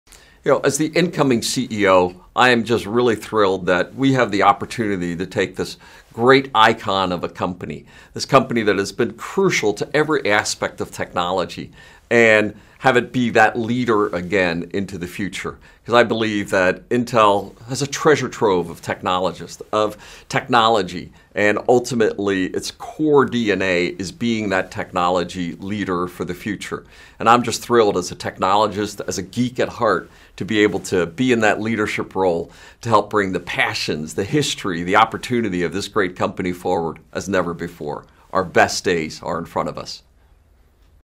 Leading Intel with CEO Pat Gelsinger
Pat Gelsinger is Intel's eighth chief executive officer in its history. Gelsinger shares his thoughts on returning to lead the company. Gelsinger is an accomplished CEO and industry veteran with more than four decades of technology and leadership experience – including 30 years at Intel, where he began his career.